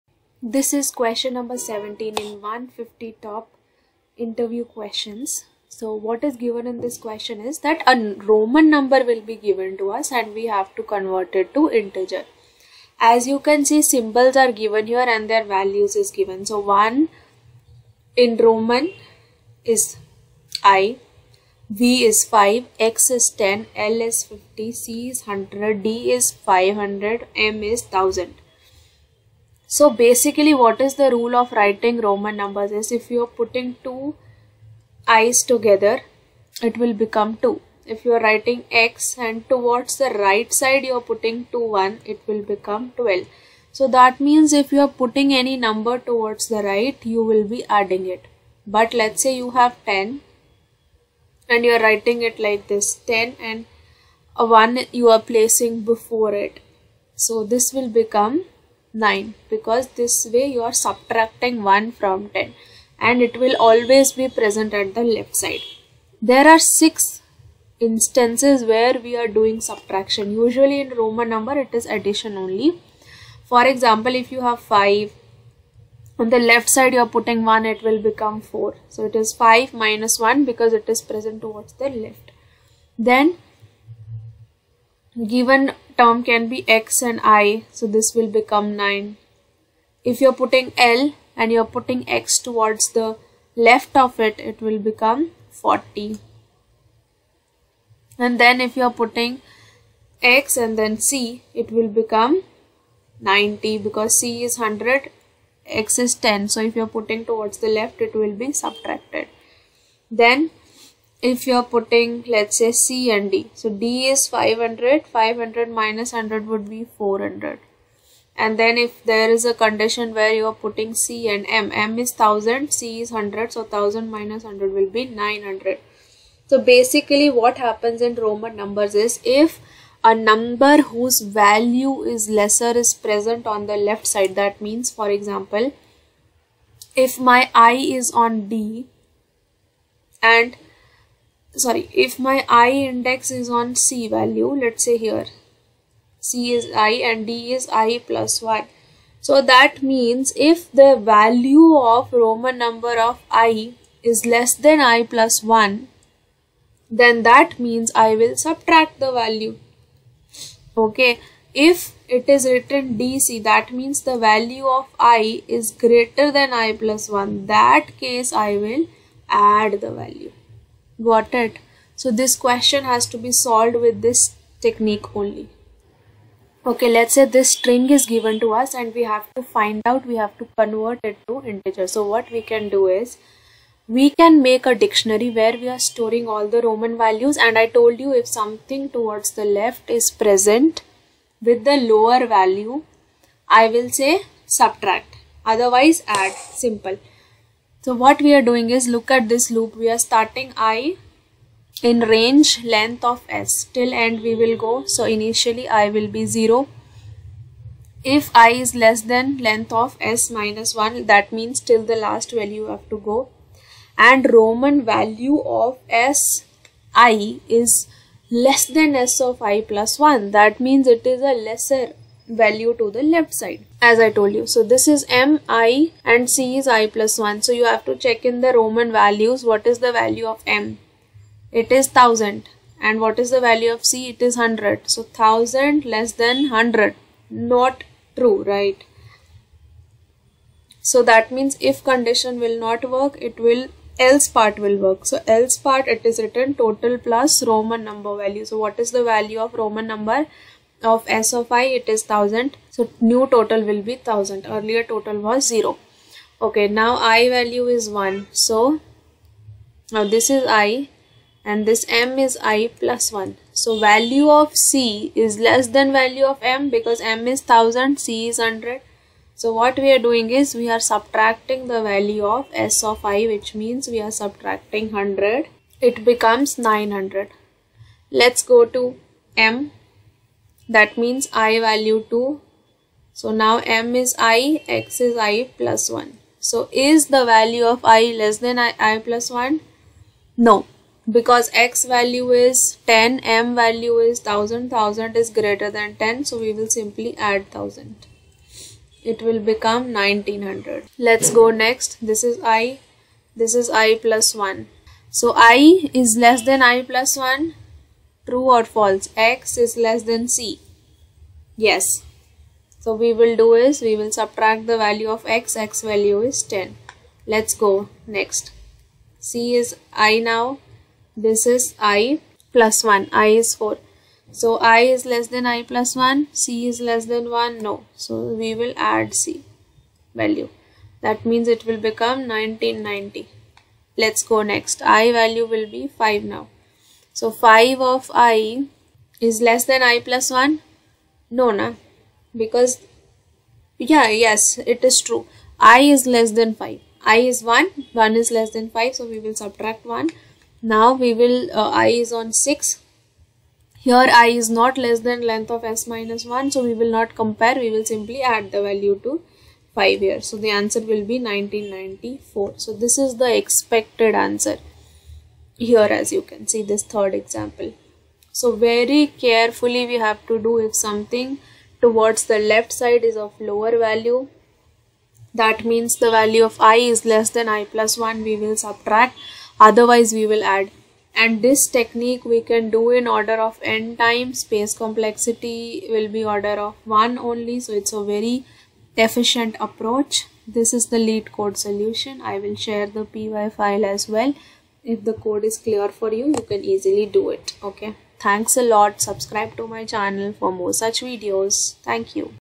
Que17 Roman To Integer | LeetCode Solution | Top 150 interview Question
This is seventeenth question from 150 interview Question Series on LeetCode.
Solution of this question is added on my github account as well on the link provided below :